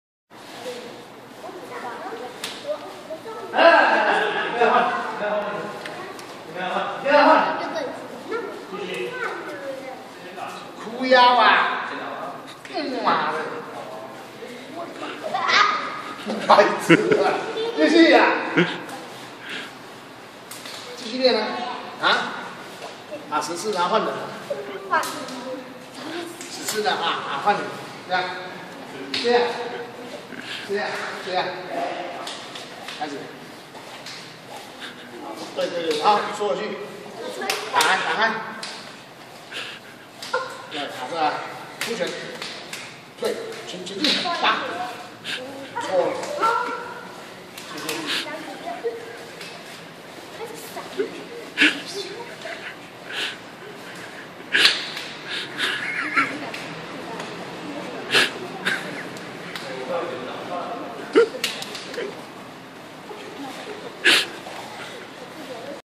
20140104揚武道館散打班教學實況3/17
好酷的小弟弟(灰衣) 哩係在教人還是打人 太口愛了吧
江正中 vs 劉子睿